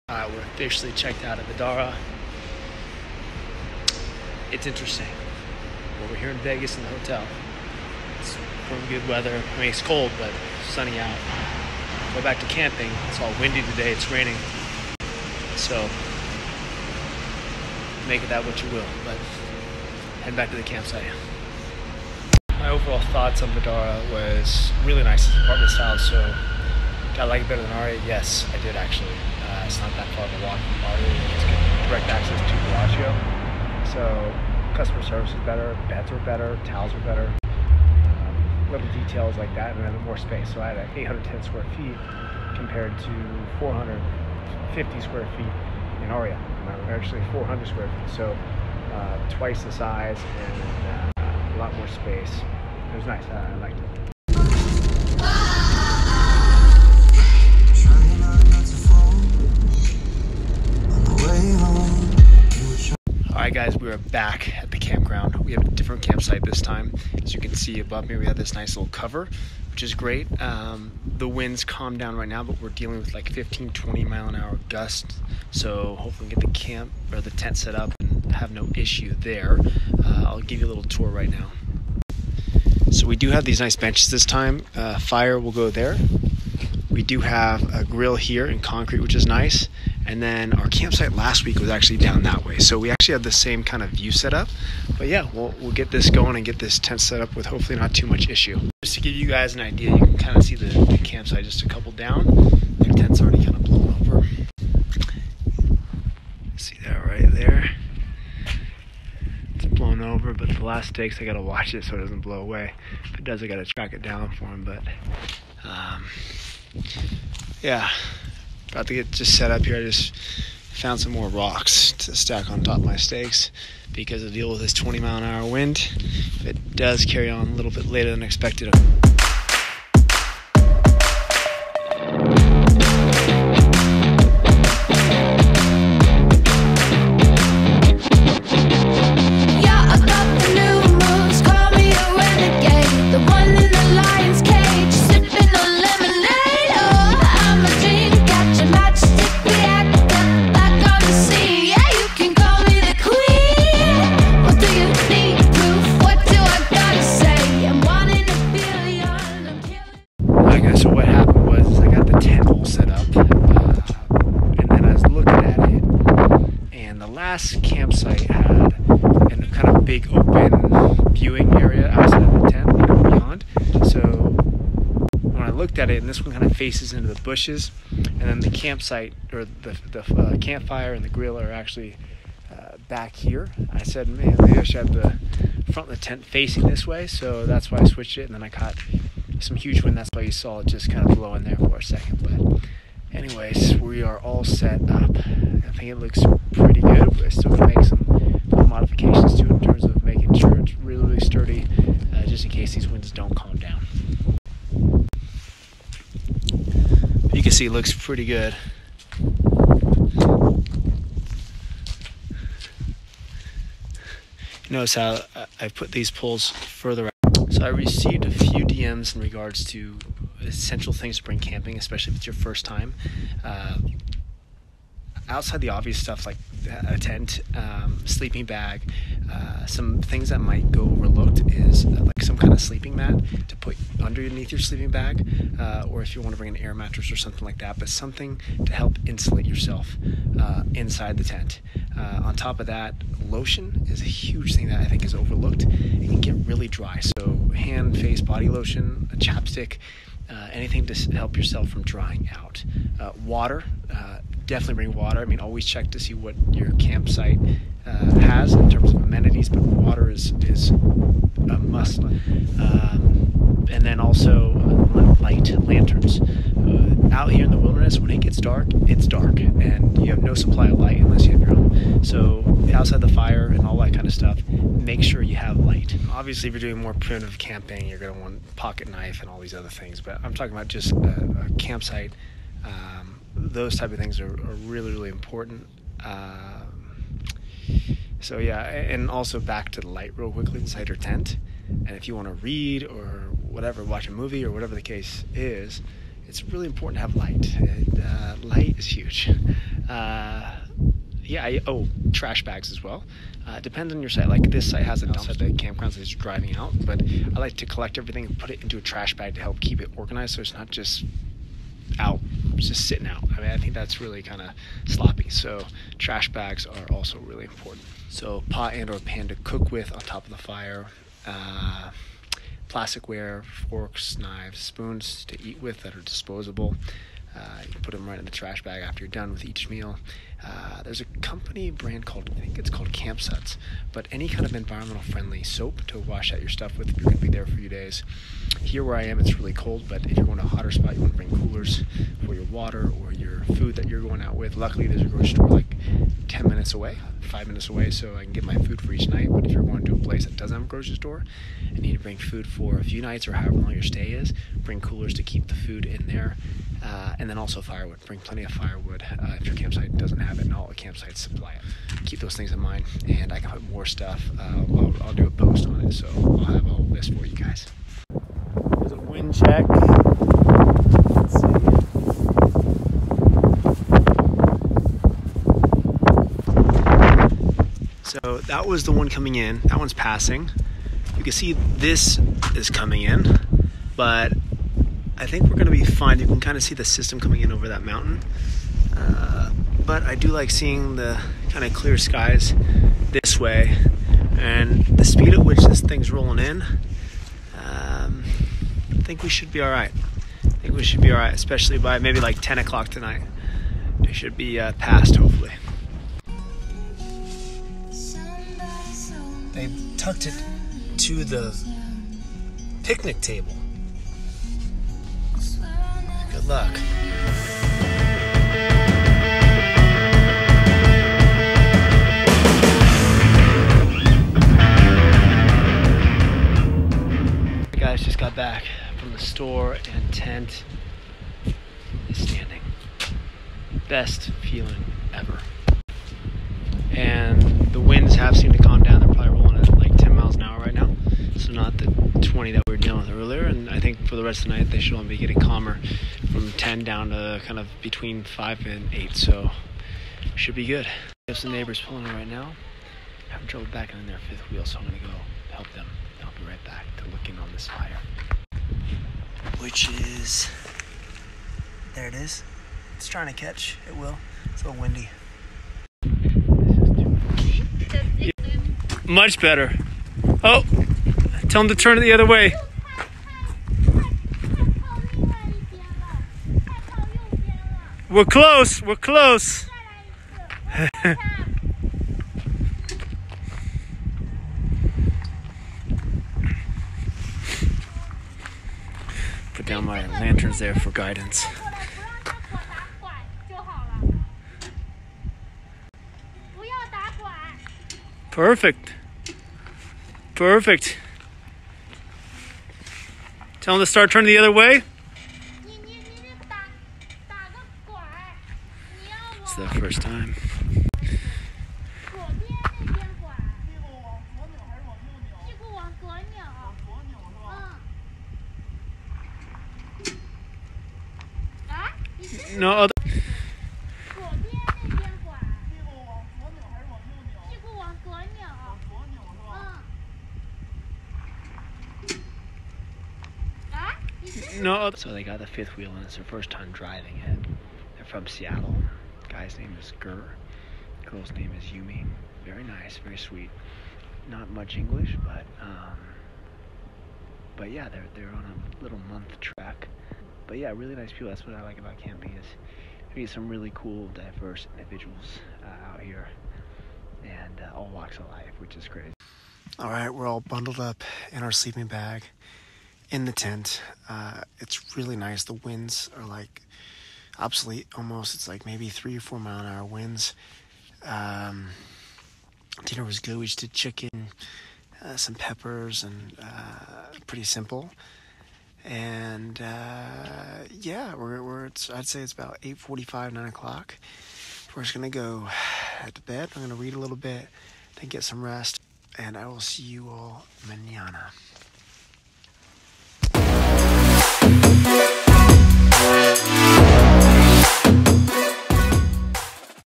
Brad heads back to camping, bye bed
After a great 3 night stay at a luxurious 5 star hotel, Vdara, he heads back to camping with no bed.

I mentioned in the video I would put my personal list of things not to forget while camping, outside the obvious things, tent, sleeping bag, etc.(recreational):
1. Always check to see your campground that you booked, and check to see which amenities it has...ie bathroom, firewood, potable water, etc.
2. Lotion, Chapstick
3. Firewood/Fire-starter
4. Pot/Pan
5. Plasticware
6. .Environmental Friendly Soap
7. Cooler (If camping in warmer temps)
8. Camping Pillow
9. Lanterns
10. Headlamp (didn't have one, but would help out a lot)
11. Sleeping pad, air mattress, or cot to put under sleeping bag
12. Sleeping bag liner (if camping in colder temperatures, (this was a life saver for me in 20 degree temps)
13. Body Towelettes to clean yourself (showers not always available, and even if your campsite has them, they are out of service due to covid), Camping showers exist as well but if camping in 3 days or less, towelettes are fine
14. Plenty of WATER
15. Hammer of some sort
16.. First Aid Kit
17. Insect Repellent
18. Trash bags
19. Reading material
20. Portable Phone Charger
21. Pocket knife of some sort is always useful